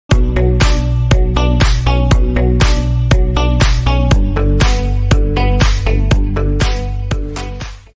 Small changes can make a big difference by Mrs Organiser
Sometimes just small changes can make a big difference to a room or area.

We always say start small don’t we? It could be a cupboard, a shelf or a landing like this.

Just clearing the clutter from this landing completely transformed this area. It feels so bright and airy doesn’t it?

Little tip for you as well to make your home feel tidier - close your doors.

Mrs Organiser offers organising and decluttering services AND home project management services across Kent & East Sussex.

I: @mrsorganiserkent2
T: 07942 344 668